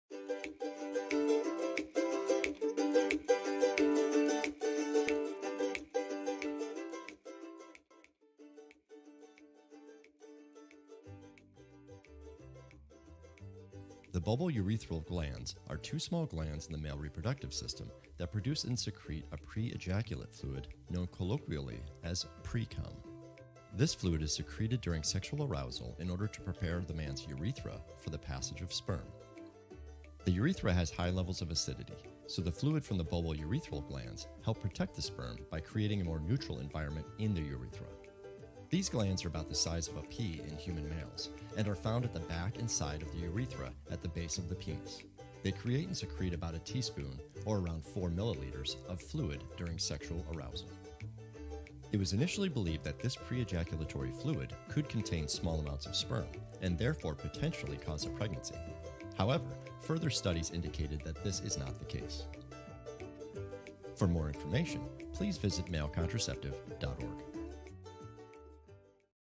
Nuts Bolts: Bulbourethral Glands
At Male Contraceptive Initiative, we know that the terminology around male contraception can be a little confusing. We created our animated film series "Nuts & Bolts" to help simplify the science!

In this episode, we provide a brief primer explaining the Bulbourethral Glands, are two small glands in the male reproductive system that produce and secrete a pre-ejaculate fluid prepare the man’s urethra for the passage of sperm.

Please visit malecontraceptive.org to learn more!